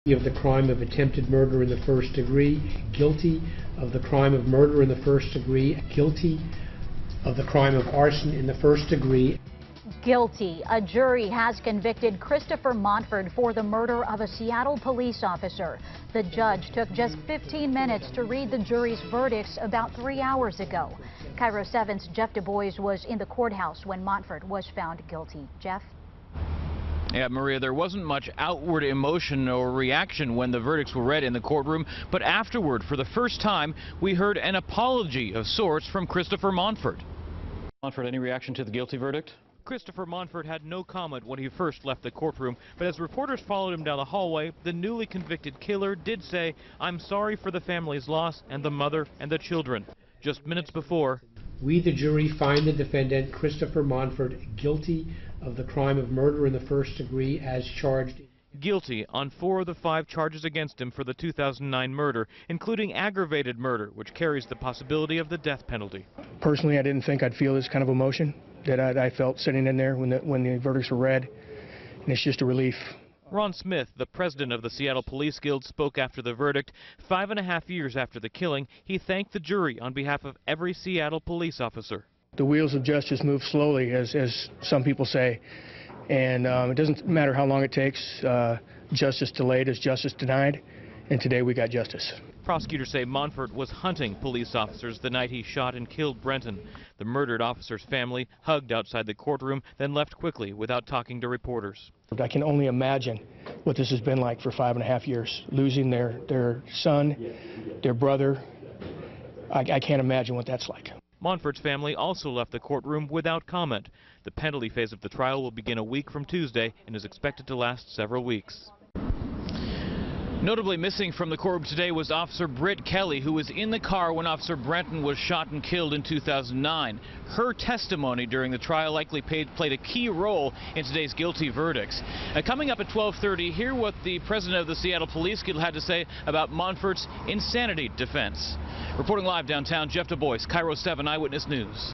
For first time, cop killer gives apology
After Christopher Monfort was convicted for killing a Seattle officer in 2009, he offered an apology of sorts.